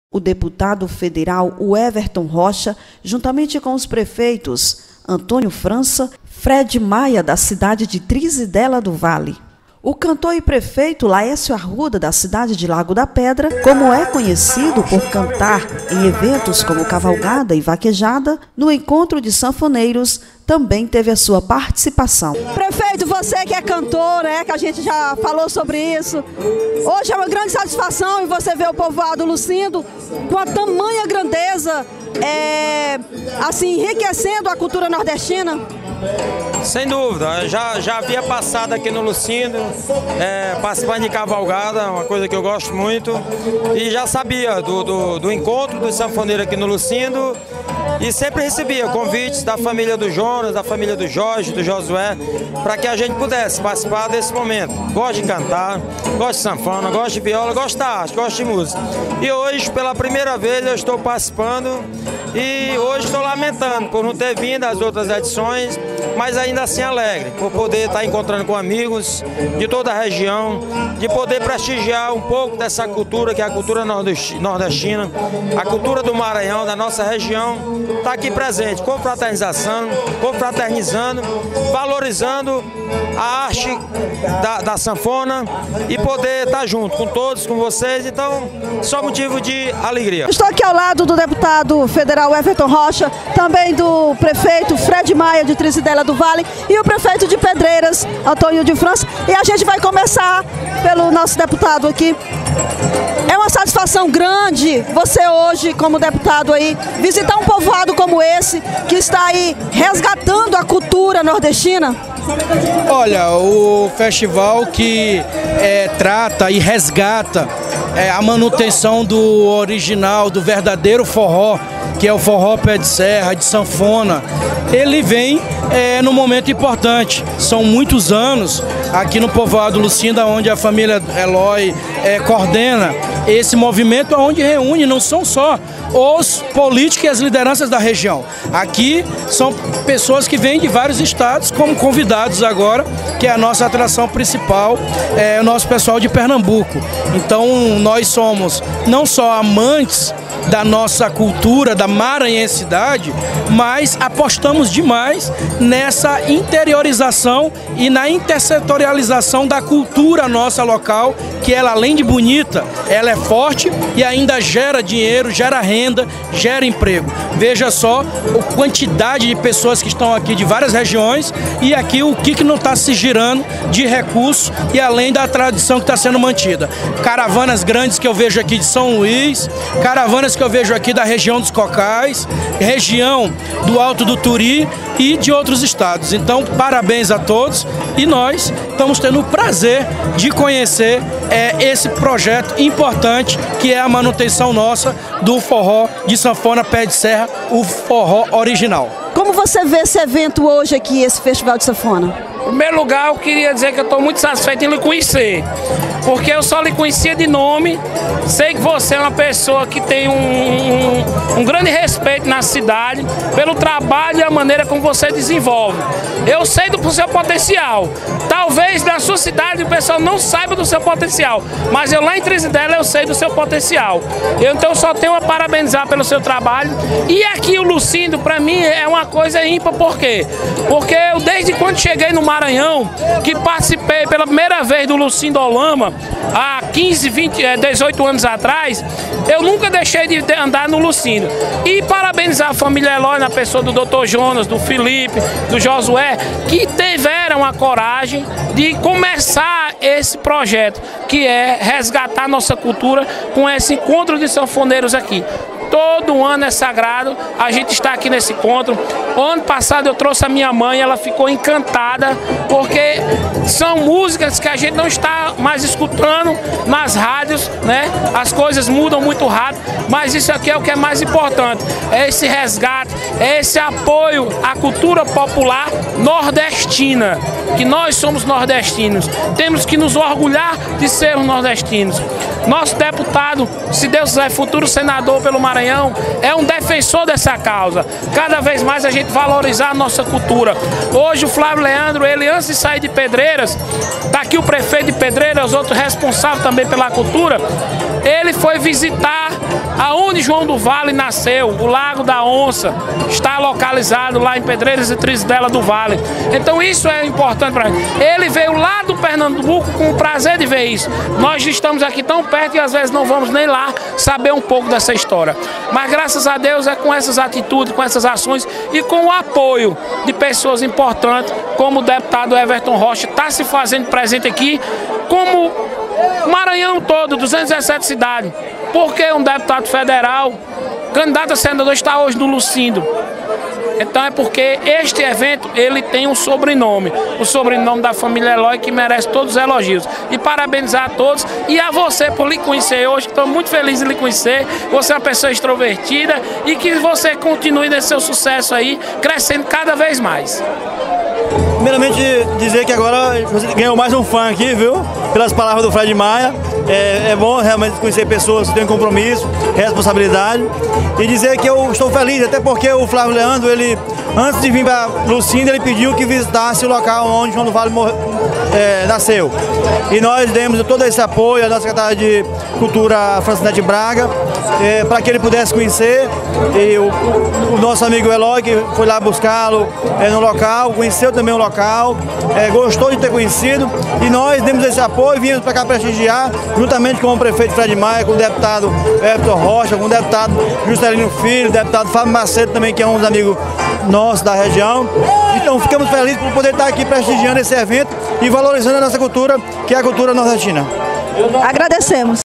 DEPUTADO WEVERTON ROCHA E PREFEITOS DA REGIÃO PRESTIGIAM FESTIVAL DE SANFONA DO LUCINDO
O Festival de Sanfona do Lucindo 2017 foi um sucesso. Sem dúvidas, uma das melhores edições realizadas. A importante comunidade de Poção de Pedras é formada por um povo acolhedor, que gosta de preservar a tradição e suas raízes culturais. Por isso, além de outros eventos realizados no Povoado, a família Eloi, na pessoa de Dr. Jonas, a cada ano traz os melhores e maiores representantes da música nordestina. Este ano foi a vez de recebermos Flávio Leandro – O Poeta Cantador, diretamente do estado de Pernambuco.